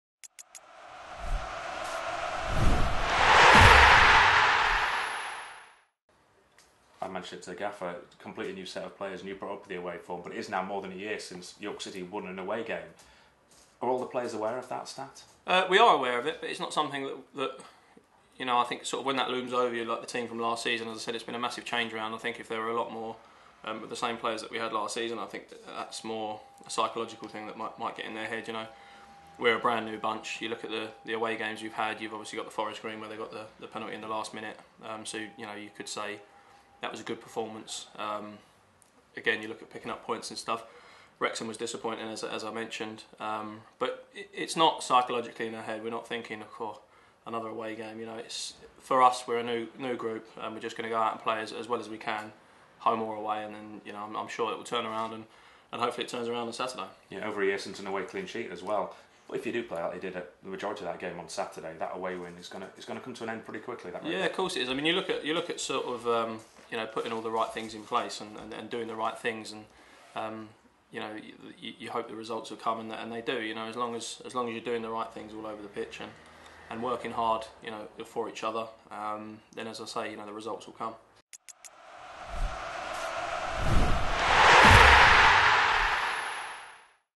Matt Fry - Pre Torquay United
Defender speaks ahead of trip to Plainmoor...